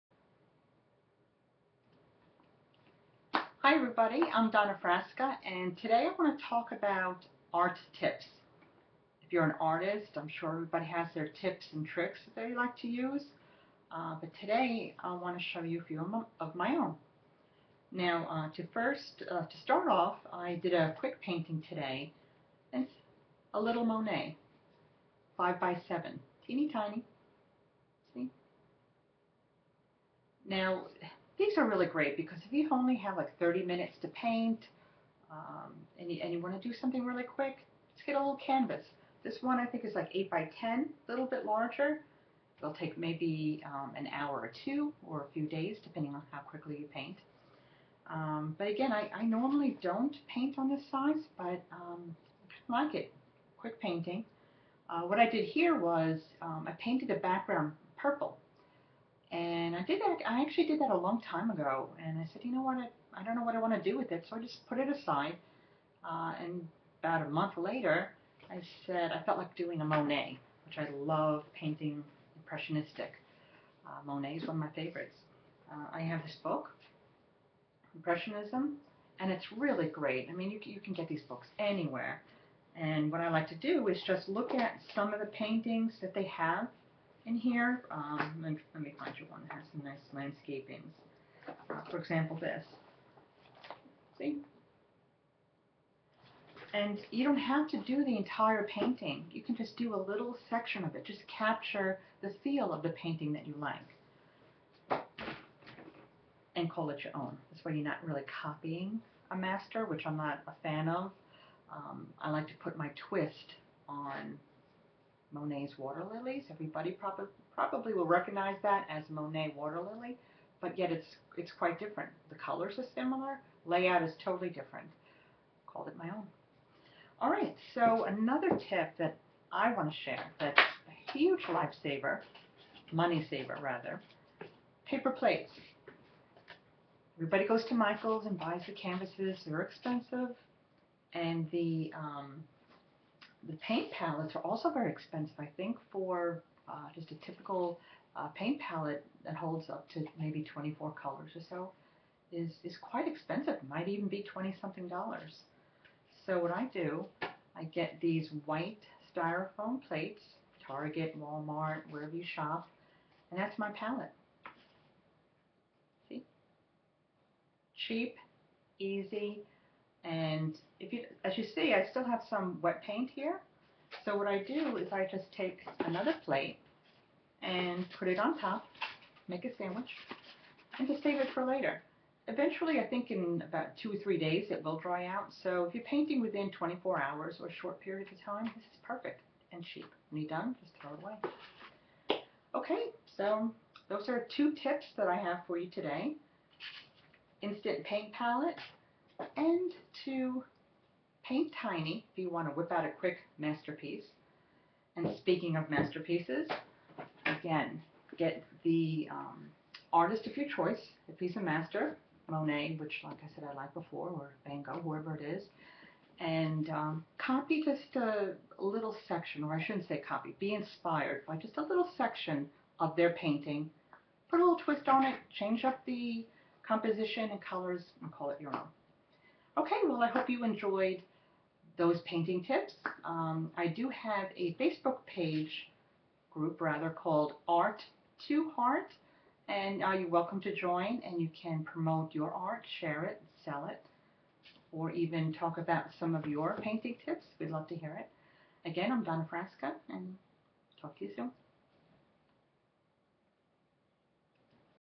Art & Painting Tips by Donna Frasca
Love art? Come listen to a few of my time and money saving tips that you can use for your next masterpeice.